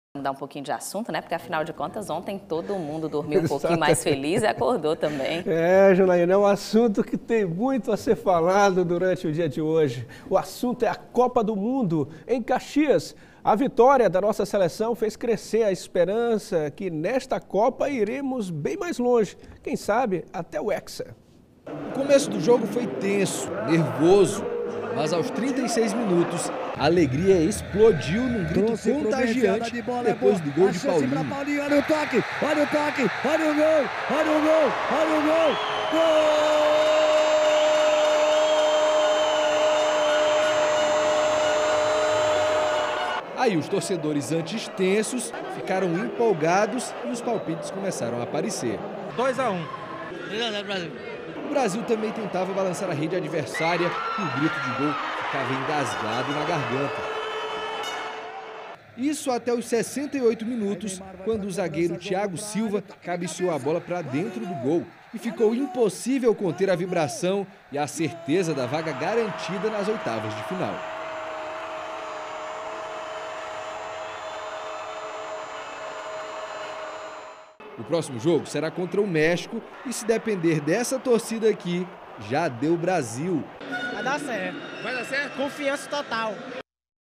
Em Caxias, a Torcida Rumo ao Hexa
População de Caxias comemora a vitória da Seleção  Brasileira.

A vitória da seleção brasileira faz crescer a esperança dos torcedores.